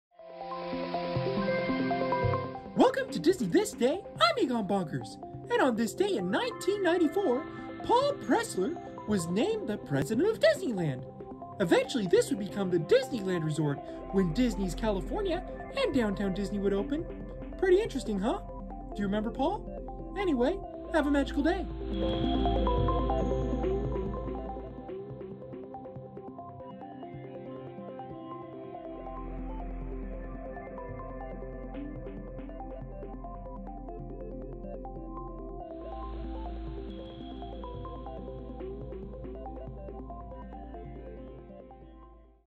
Paul Pressler - DISNEY THIS DAY - November 7, 1994
On this day in 1994, Paul Pressler is named the new President of Disneyland, and ultimately the Disneyland Resort when Disney's California Adventure and Downtown Disney opens.